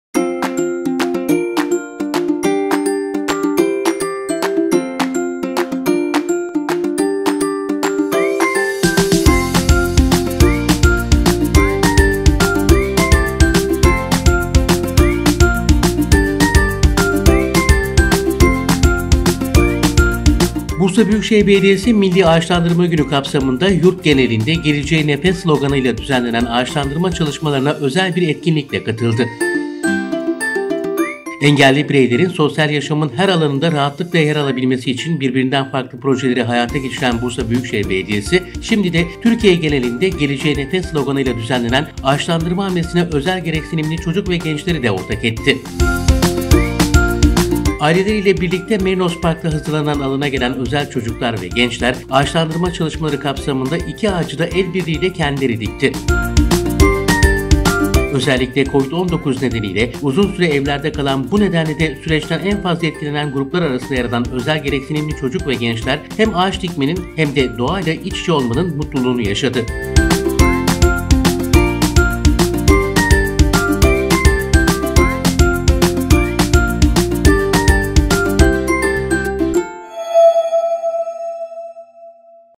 Geleceğe özel bir nefes
Bursa Büyükşehir Belediyesi Milli Ağaçlandırma Günü kapsamında yurt genelinde ‘Geleceğe Nefes’ sloganıyla düzenlenen ağaçlandırma çalışmalarına özel bir etkinlikle katıldı. Özel gereksinimli çocuk ve gençler de Merinos Park’ta ağaç dikerek, geleceğe bir nefes olmanın ayrıcalığını yaşadı.

Engelli bireylerin sosyal yaşamın her alanında rahatlıkla yer alabilmesi için birbirinden farklı projeleri hayata geçiren Bursa Büyükşehir Belediyesi, şimdi de Türkiye genelinde ‘Geleceğe Nefes’ sloganıyla düzenlenen ağaçlandırma hamlesine özel gereksinimli çocuk ve gençleri de ortak etti. Aileleriyle birlikte Merinos Park’ta hazırlanan alana gelen özel çocuklar ve gençler, ağaçlandırma çalışmaları kapsamında iki ağacı da el birliğiyle kendileri dikti.

Özellikle covid 19 nedeniyle uzun süre evlerde kalan bu nedenle de süreçten en fazla etkilenen gruplar arasında yer alan özel gereklisinimli çocuk ve gençler, hem ağaç dikmenin hem de doğa ile iç içe olmanın mutluluğunu yaşadı.